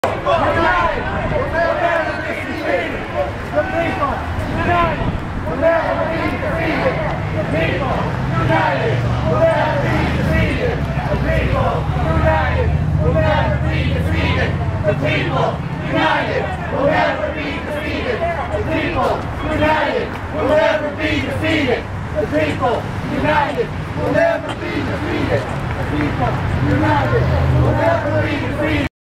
Occupy Regina - The Plains
The "people united", in front of the now demolished Plains Hotel at Victoria Ave. and Albert St. on 22nd October, 2011